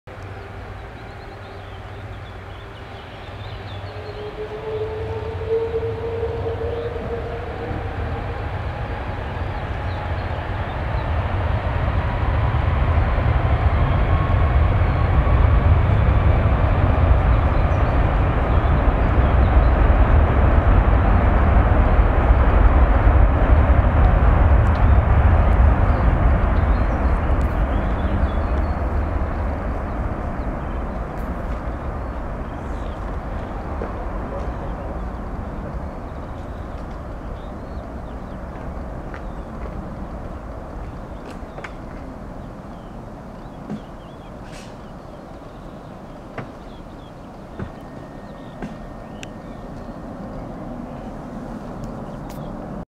Onurair Airbus A330 take off at Nuremberg, (NUE/EDDN)
6 Mai 2018 / Onurair / Airbus A330-200 / TC-OCN

Video by Daniel :D